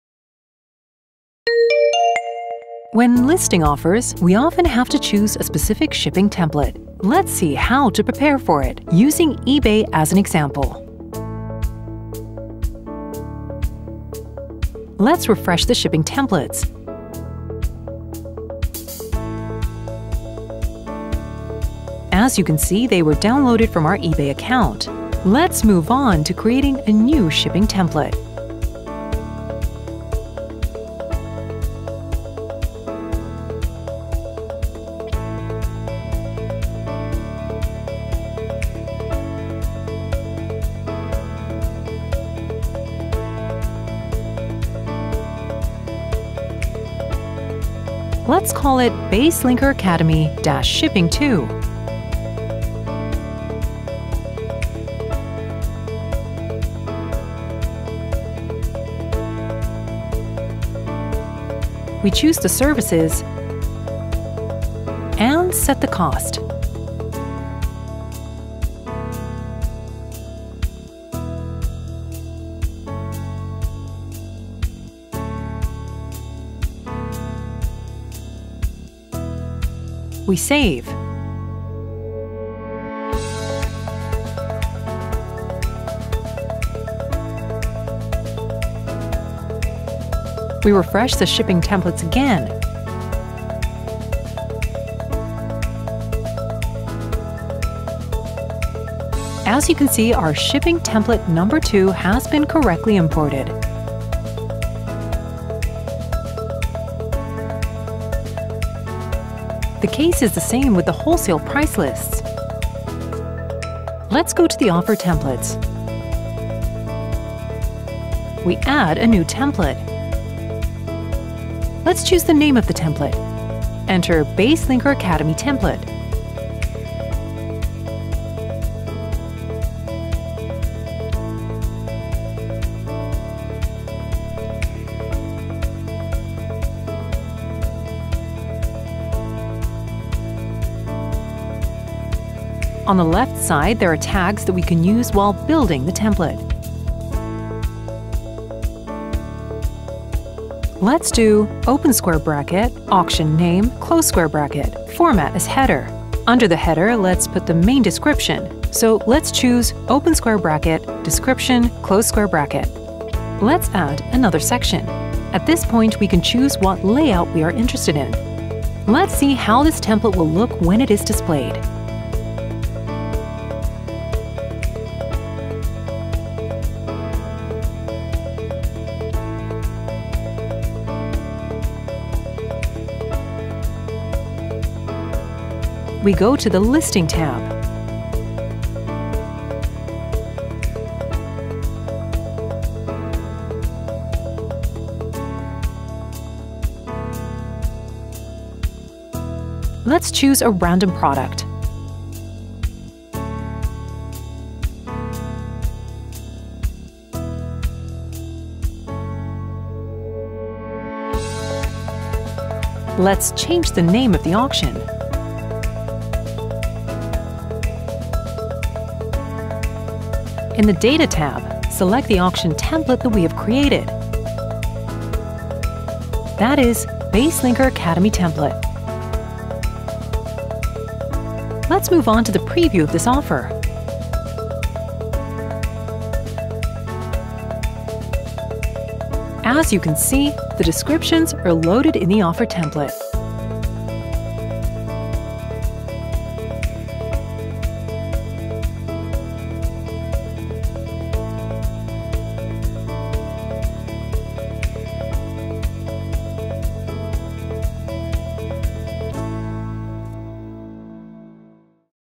Offer templates and shipping templates | BaseLinker Academy
Offer templates and shipping templates available in the BaseLinker system will not only unify the appearance of the offers you publish, but also significantly speed up their listing and facilitate data editing during the auction. We show you how to create and configure shipping templates and offer templates in a few simple steps.